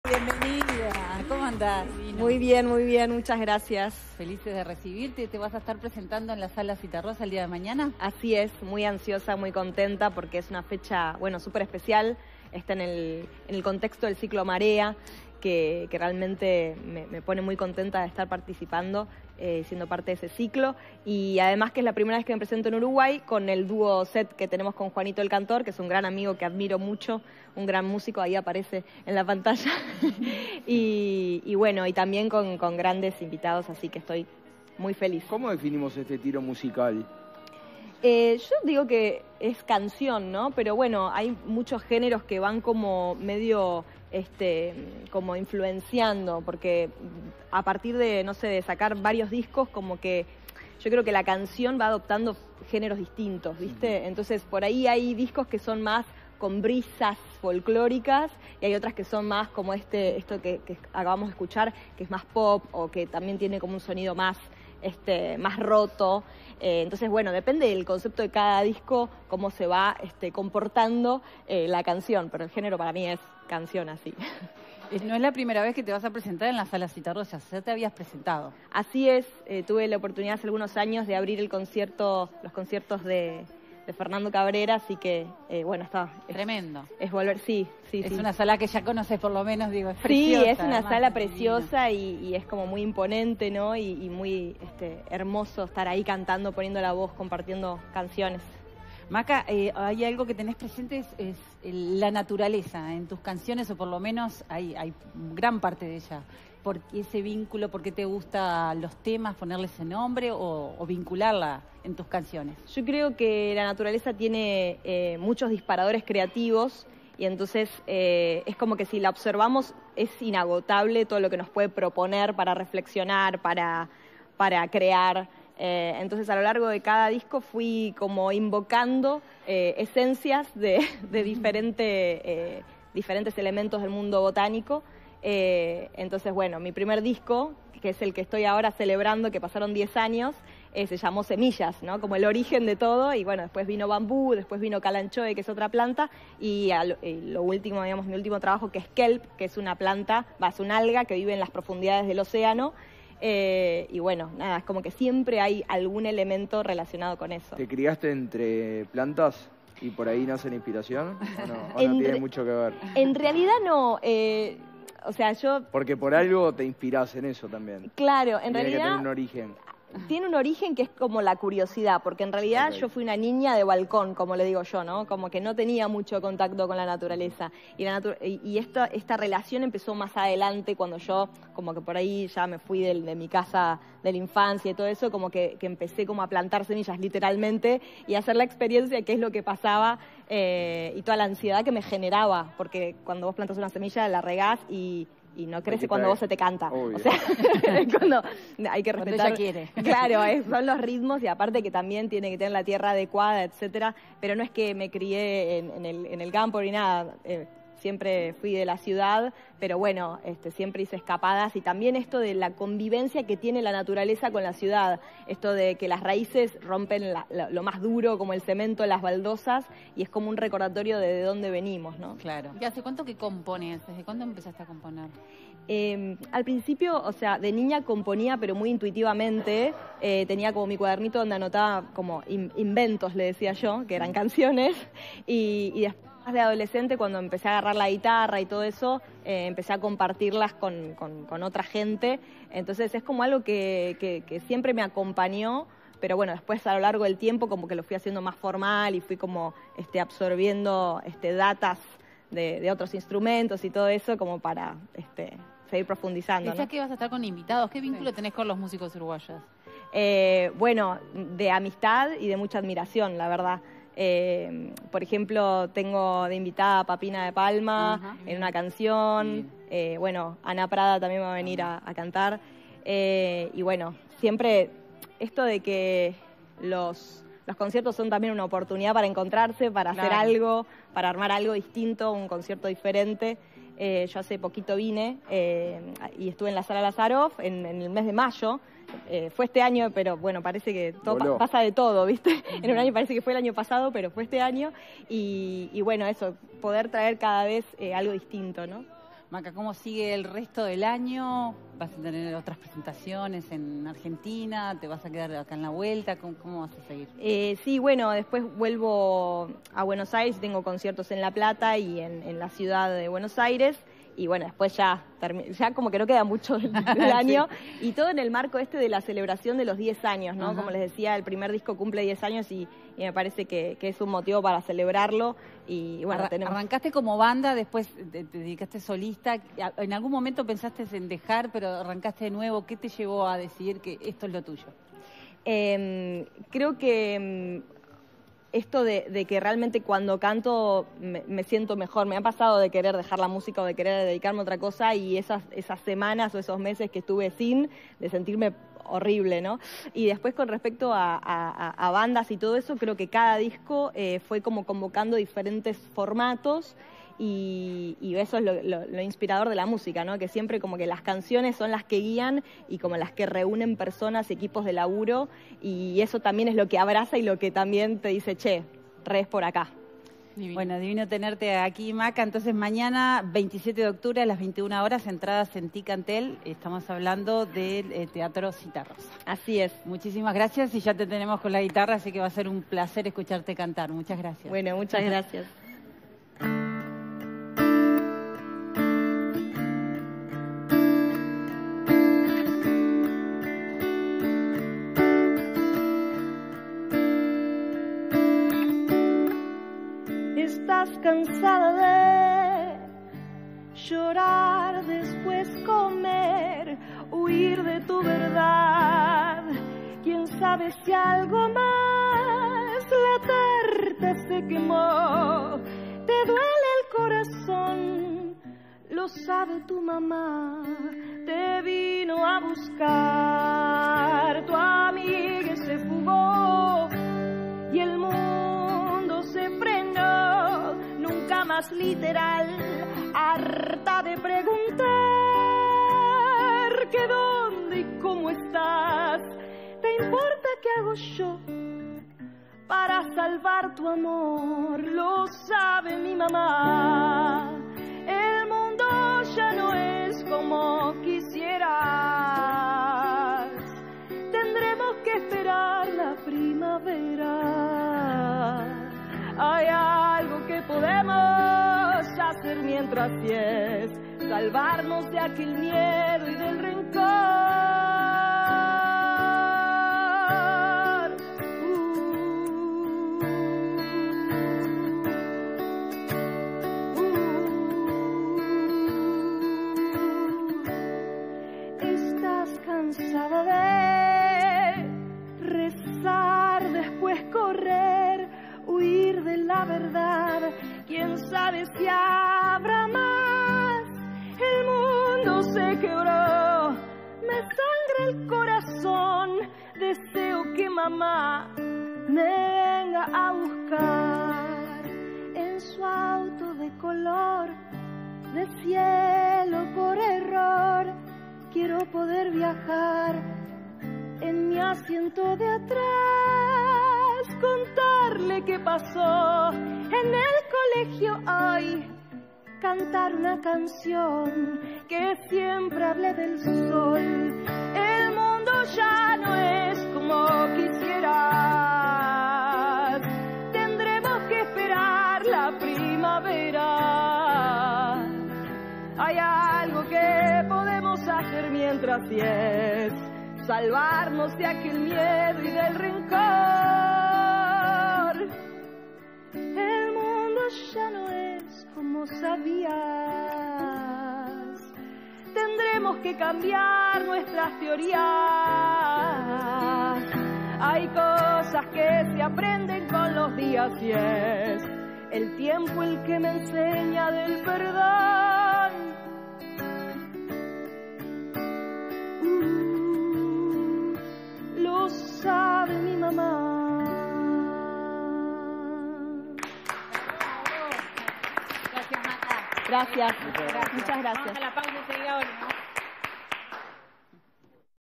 Maca Mona Mu: su búsqueda de identidad dentro de la escena musical
Recibimos en el estudio a Macarena Muñoz, mejor conocida como Maca Mona Mu. Se presenta por primera vez en la Sala Zitarrosa el 27 de octubre.